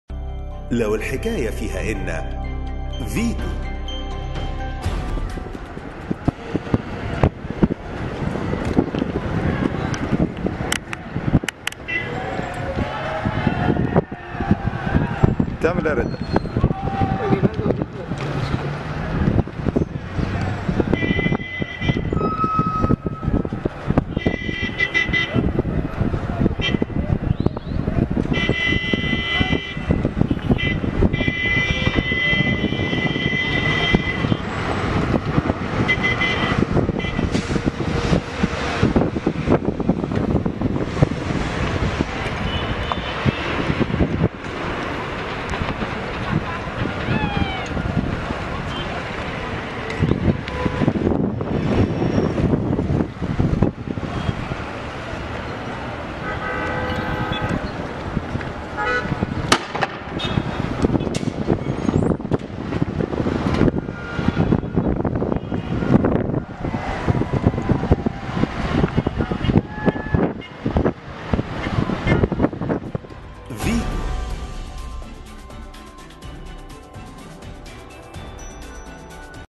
طلاب «الإرهابية» بالأزهر يهاجمون قوات الشرطة بالحجارة
هاجم عدد من طلاب جماعة الإخوان الإرهابية، قوات الشرطة المتمركزة خارج المدينة الجامعية للأزهر، اليوم الجمعة.